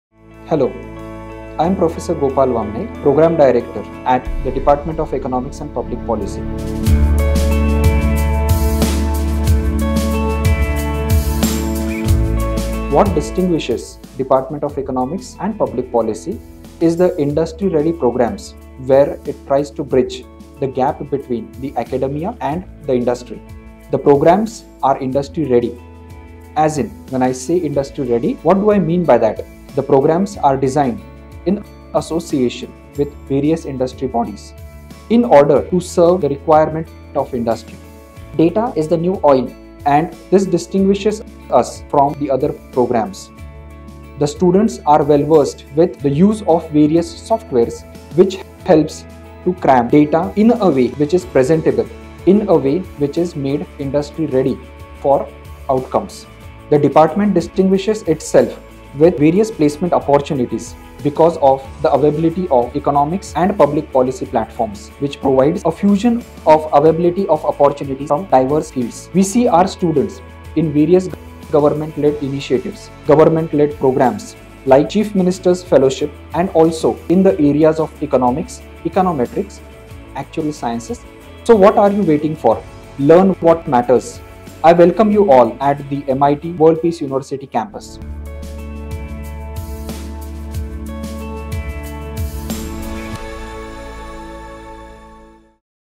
Unlock Your Future in Economics and Public Policy at MIT-WPU with Prof. Gopal Wamane
Prof. Gopal Wamane, the Programme Director of the Department of Economics and Public Policy at MIT-WPU, invites you to explore how their innovative, industry-ready programmes bridge the gap between academia and the professional world. Designed in collaboration with various industry bodies, these programmes equip students with the latest data analysis skills, ensuring they are prepared for diverse career opportunities in government, economics, econometrics, and actuarial sciences. Join us at MIT World Peace University and transform your potential into success.

Learn. Lead. Transform.